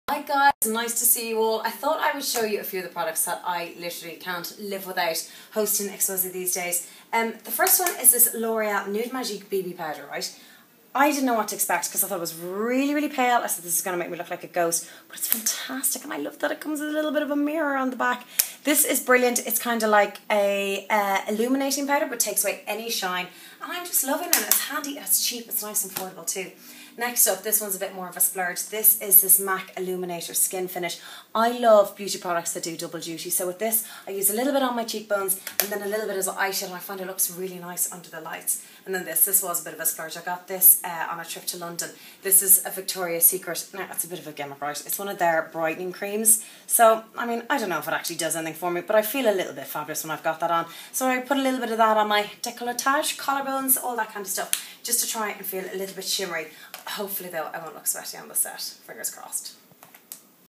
Xpose's Karen Koster's Make Up Bag
We caught up with Karen as she got ready for the show. She very kindly talked us through her favorite must have make up products at the moment!